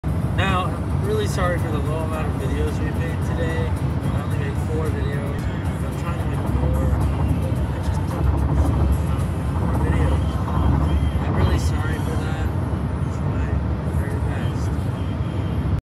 I’m sorry for the low number of videos made today.
I only made 4 videos today and I’m trying to make more videos.